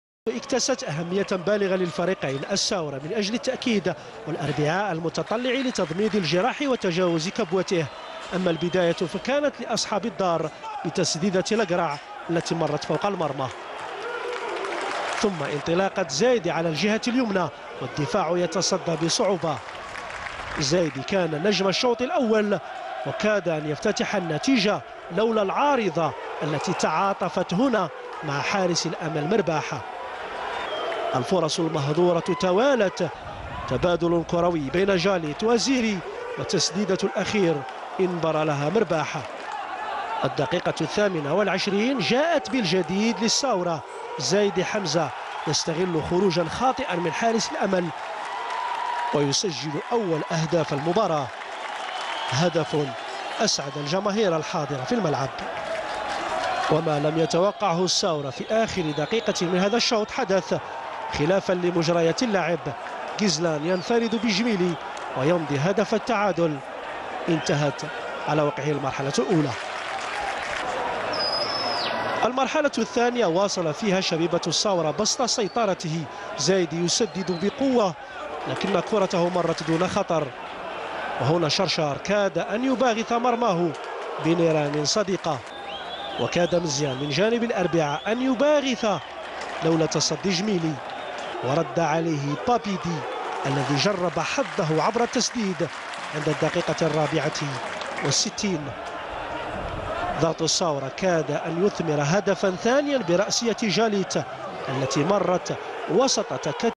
gardien de but gaya merbah rcarbaa vs jssaoura 2015/2016
قاية مرباح كان في يومه وتصدى لعدة فرص امام شبيبة الساورة